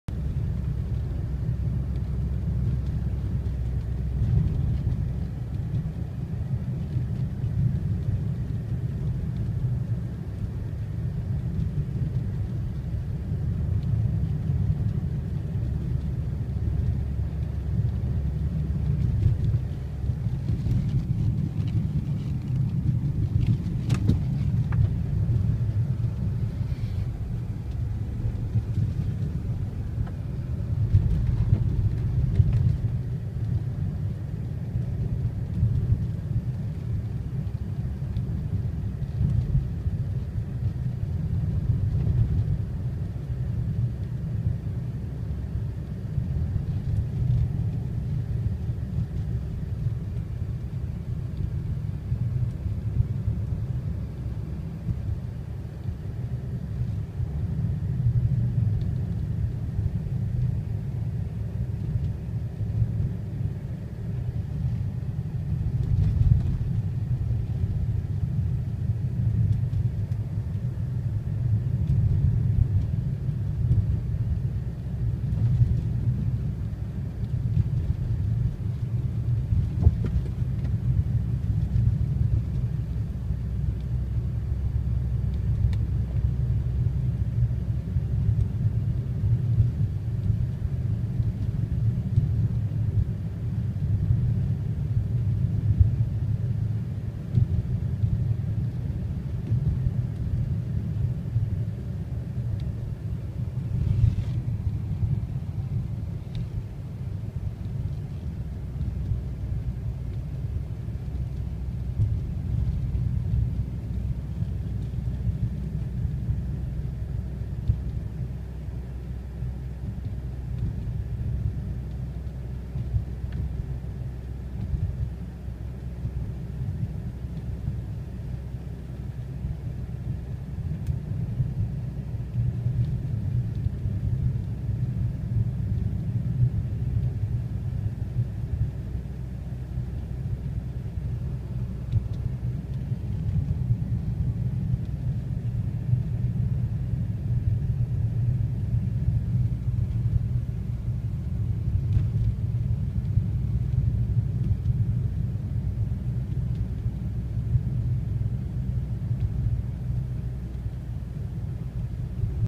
Beautiful Snow Capped Trees
My drive into work after the Midwest's Snowstorm Linus dumped 16" on us! It's like driving into Narnia!